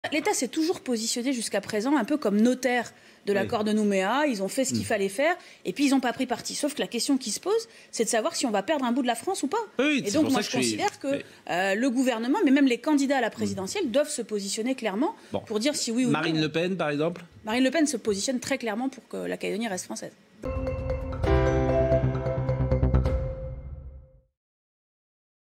Sonia Backes frappe un grand coup médiatique
La présidente de la province Sud a profité de son séjour à Paris pour participer à l’émission « L’heure des pros" de CNEWS. Une intervention remarquée qui lui a notamment valu la proposition de Jacques Séguela d’apporter son aide à la campagne des Loyalistes. Rappelons que le célèbre communiquant, vice-président de l’agence de publicité Havas, est à l’origine du slogan « La Force Tranquille » qui avait contribué à la victoire de François Mitterrand en 1981.